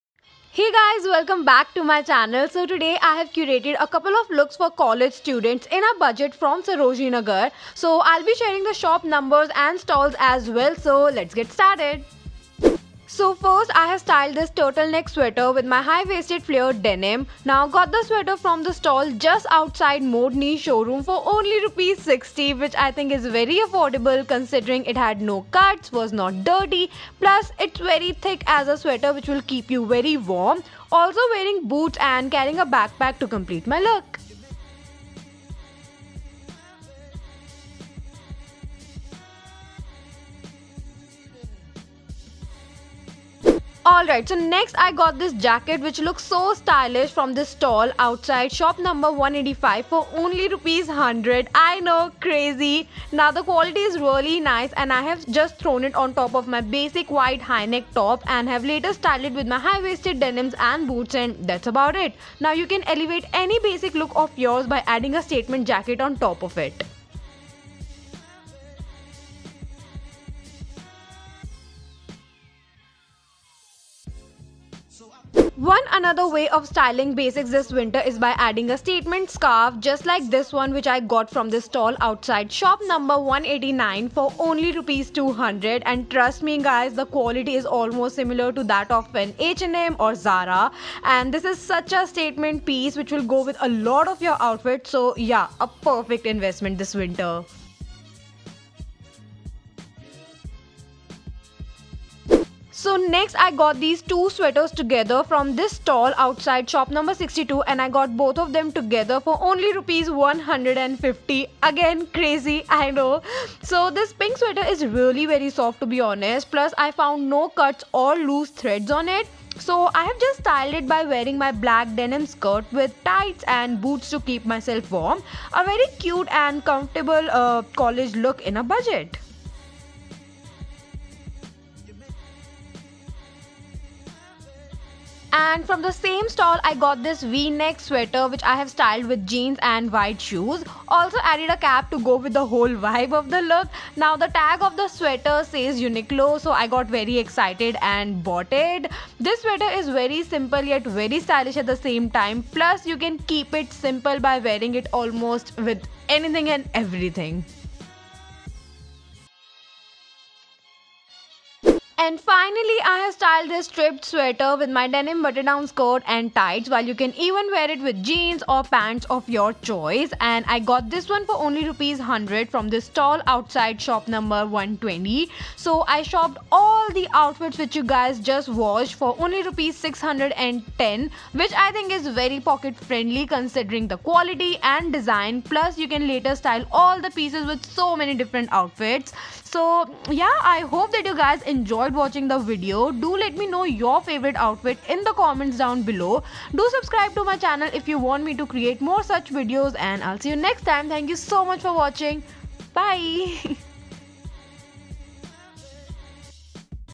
Affordable Winter College Outfits from Sarojini Nagar| SONIA GARG
Sharing an affordable College Winter Lookbook in this one. Let me know your favourite look in the comments down below.
CAMERA USED- GOOGLE PIXEL 2. VIDEO EDITED BY ME ON PREMIERE PRO CS6.
. Xoxo.